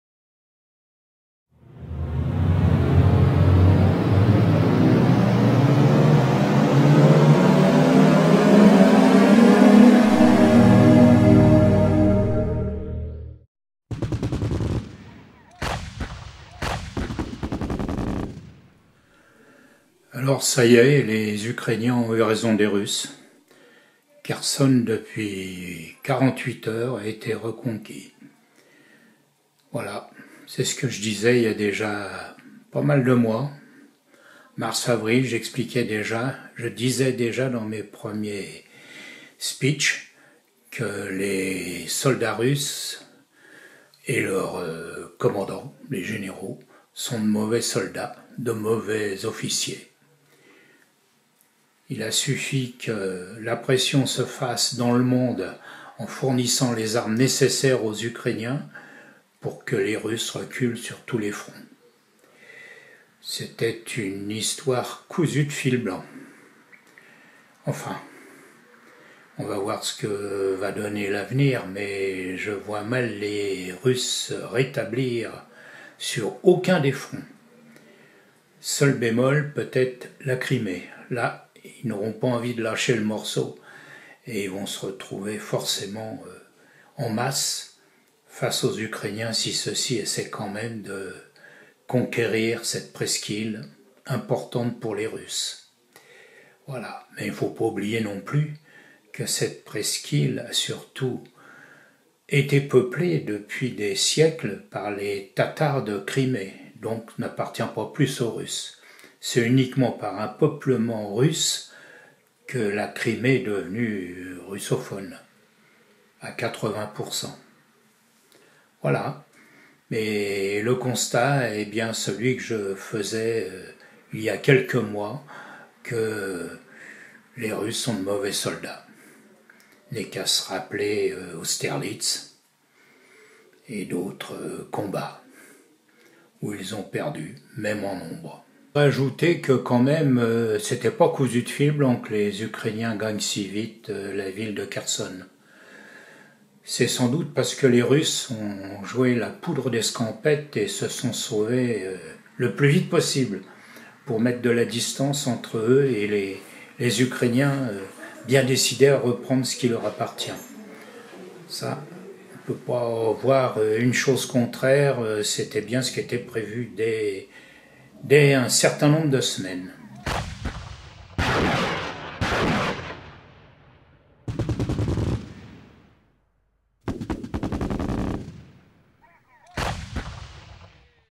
La Poudre d'escampette
Mauvais soldats, plus mauvais officiers, plus généraux nuls et le cocktail est parfait pour perdre sur tous les fronts... Alors dans ce cas là reste à plier bagage sans combattre, c'est l'avenir de l'armée Russe qui a tant fait peur pendant 45 ans... Et qui aujourd'hui abandonne la ville de Kherson devenue Russe par la volonté de Poupoutitine Ier... Quel pauvre crétin que ce tzar de pacotille... Il fait penser à Mussolini la grenouille qui voulait se faire plus grosse que le boeuf... Il ne suffit pas d'avoir une grande gueule pour conquérir les états adjacent, il faut l'armée qui va avec...